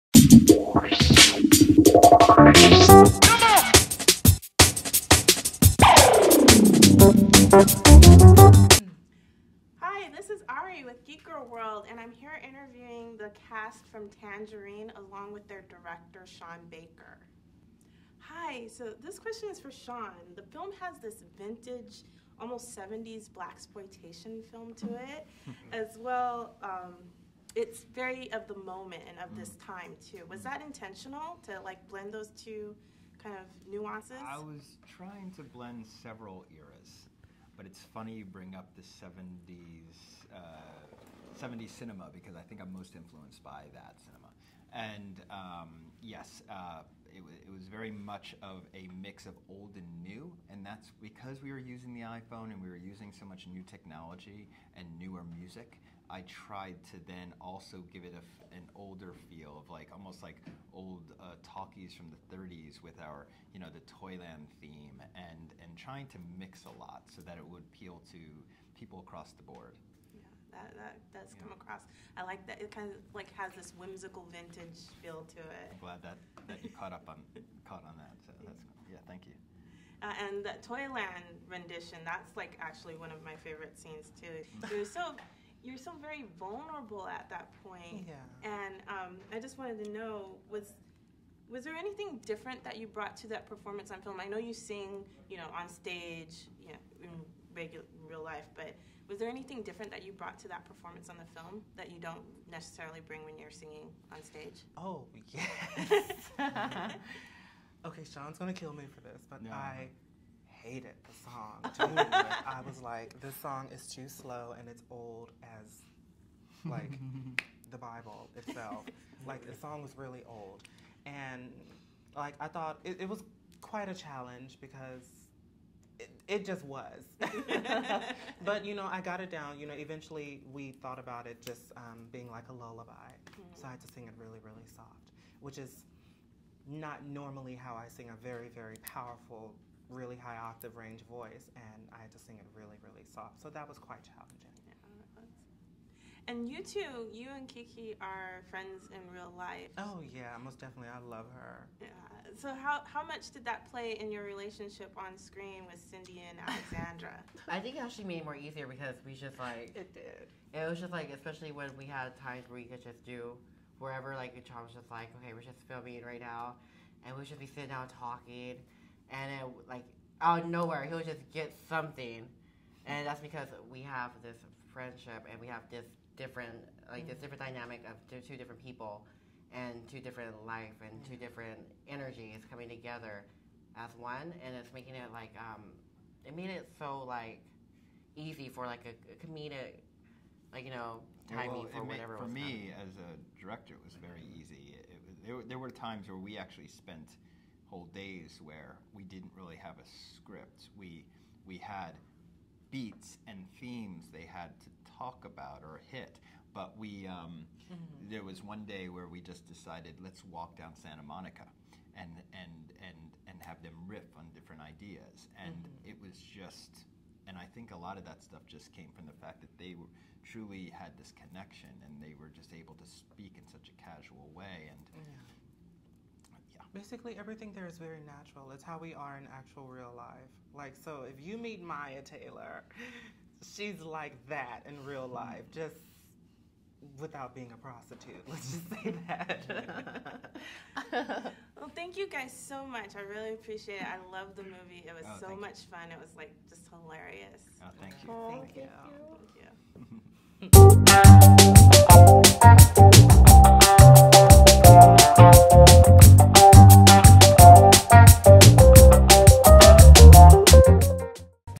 Interview Special: Tangerine
Using the streets of LA as it’s backdrop, "Tangerine" is a nonstop ride that you will be sad to leave when it’s over.

"Tangerine" tells the story of Sin-Dee Rella, a transgender prostitute who is fresh out of jail on Christmas Eve, and her best friend Alexandra (also a transgender prostitute) who hits her with some disturbing news; her fiance, a pimp named Chester, has been cheating on her with “real fish” (slang for biological women). Sin-Dee then goes on a city wide rampage to find the “fishy” girl her would-be husband has been screwing around with behind her back. Let’s just say, it isn’t rainbows and unicorns when the two meet.

The entire film was shot on iPhone 5S, giving it this intensely personal and voyeuristic look into both Sin-Dee and Alexandra’s lives as they go about the day separately with two very different goals in mind. Director Sean Baker (Prince of Broadway, Starlet), gives us intimate portrait of friendship, how people can be tested in life, and the relationships that really matter to each of us at the end of the day.

As poignant as this movie was, it was also rip-roaring hilarious. There were very few slow moments in the story, and over all this movie was just fun and enjoyable to watch. The one tiny thing that bothered me was that the ending seemed a little too sudden and choppy. I think the story could have been wrapped up a little neater. Then again, there was nothing “neat” about the lives of Sin-Dee and Alexandra in this film. It was very raw and very real, along with the fun “whimsical vintage” blaxplotation look and feel to it all. So, having an ending wrapped in a pretty neat little bow just would not have been "Tangerine".

GeekGirl Ari got the chance to sit down with Sean Baker and stars, Mya Taylor and Kitana Kiki Rodriguez, to talk about the film and if it really was as fun to make as it looked.

"Tangerine" is in theaters July 10, 2015.